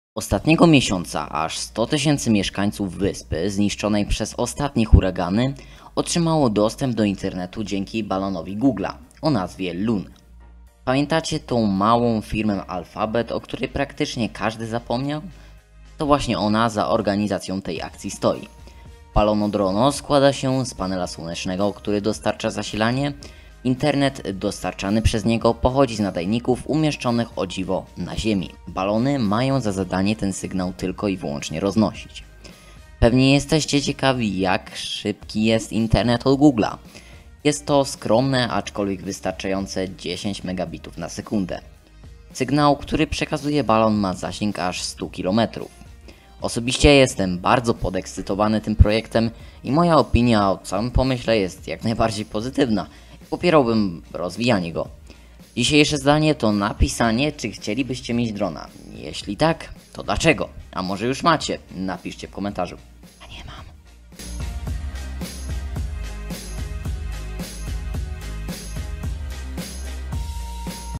INTERNET dostarczany BALONEM?
Wyobraź sobie, że kiedyś nad twoim domem będzie latał balon dostarczający internet... Google dzięki projektowi Loon rozpowszechnia dostęp do sieci, tam gdzie go nie ma!

Źródła:
Brak internetu!
Super szybki kot!